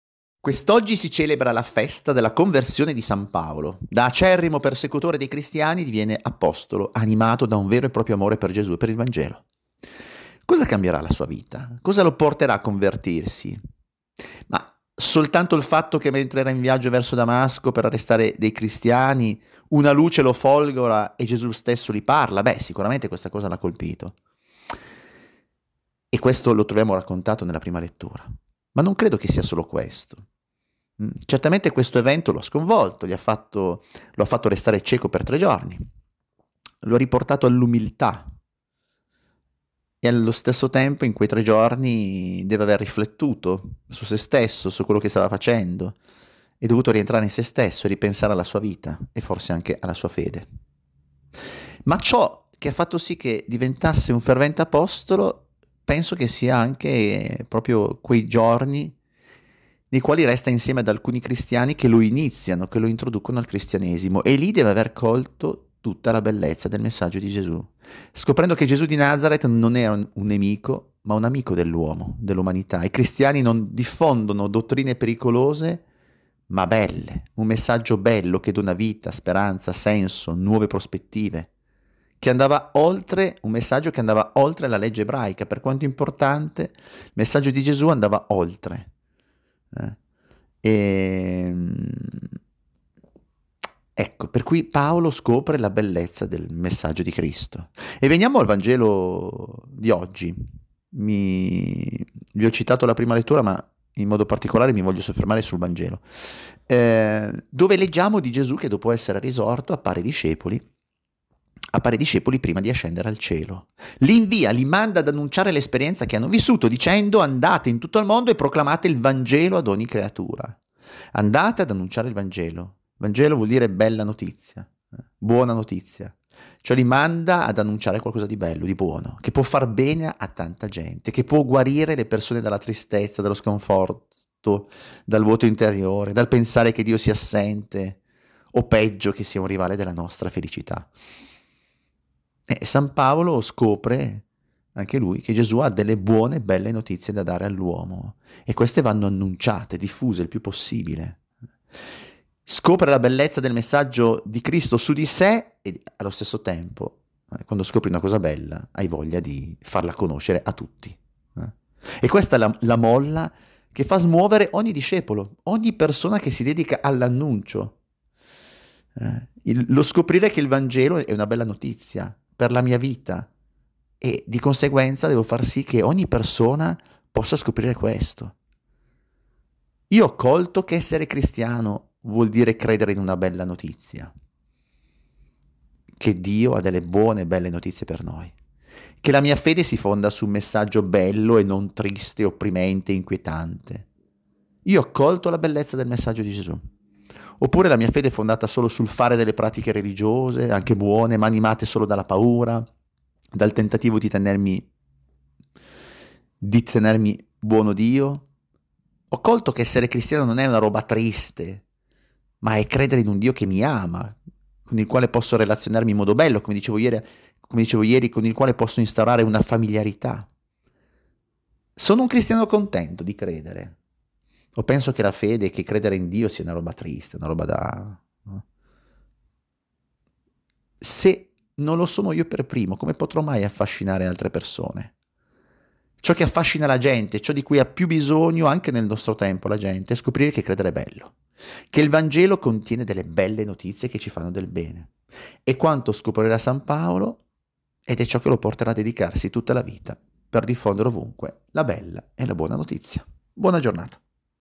Contenti di credere - don Paolo Quattrone
Commento di don Paolo Quattrone - sacerdote della diocesi di Aosta, parroco di Bard, Donnas, Hône e Vert.
Mercoledì 25 Gennaio

Oggi si celebra la festa della conversione di san Paolo, da acerrimo persecutore dei cristiani diviene apostolo animato da un vero e proprio amore per Gesù e per il Vangelo. Cosa cambierà la sua vita, cosa lo porterà a convertirsi? Il fatto che mentre era in viaggio verso Damasco per arrestare dei cristiani una luce lo folgora e Gesù stesso gli parla? Così come ci racconta la prima lettura? Non credo che sia solo questo. Certamente questo evento lo ha sconvolto e il fatto di restare cieco per tre giorni lo ha riportato all’umiltà e allo stesso tempo lo ha costretto a riflettere, a rientrare in se stesso e a ripensare alla sua vita. Ma ciò che ha fatto sì che diventasse un fervente apostolo è il fatto che per alcuni giorni resta insieme ad alcuni cristiani che lo iniziano, lo introducono al cristianesimo e lì deve aver colto tutta la bellezza del messaggio di Gesù scoprendo che Gesù di Nazaret non è un nemico ma un amico dell’uomo, dell’umanità, i cristiani non diffondono dottrine pericolose ma un messaggio bello, che dona vita, senso, nuove prospettive, che andava oltre la Legge ebraica di cui Paolo era attento osservante e difensore. E veniamo al Vangelo di oggi, dove leggiamo di Gesù che dopo essere risorto appare ai discepoli e prima di ascendere al cielo li invia, li manda ad annunciare l’esperienza che hanno vissuto dicendo: “Andate in tutto il mondo e proclamate il Vangelo ad ogni creatura”. Andate ad annunciare il Vangelo, cioè una bella, una buona notizia che può far bene a tanta gente, che può guarire le persone dalla tristezza, dal vuoto interiore, dal pensare che Dio sia assente o peggio che un rivale della nostra felicità. San Paolo scopre che Gesù ha buone e belle notizie da dare all’uomo e queste vanno annunciate, diffuse il più possibile. E’ questa la molla che fa smuovere ogni discepolo, ogni persona che si dedica all’annuncio, scoprire che il Vangelo è una bella notizia per la mia vita e devo far sì che anche altri lo scoprano! Io ho colto che essere cristiano vuol dire credere in una bella notizia? Che la mia fede si fonda su un messaggio bello e non triste, inquietante, opprimente? Io ho colto la bellezza del messaggio di Gesù? Oppure la mia fede è fondata solo sul fare delle pratiche religiose, anche buone ma animate solo dalla paura, dal tentativo di tenermi buono Dio? Ho colto che essere cristiano non è una roba triste ma è credere in un Dio che mi ama, con il quale posso relazionarmi in modo bello? Sono un cristiano contento di credere? Se non lo sono io per primo come potrò mai affascinare altre persone? Ciò che affascina la gente, ciò di cui ha più bisogno, anche nel nostro tempo, è scoprire che credere è bello, che il Vangelo contiene delle belle notizie che ci fanno del bene. E’ quanto scoprirà san Paolo e che lo porterà a dedicarsi tutta la vita per diffondere ovunque la bella notizia.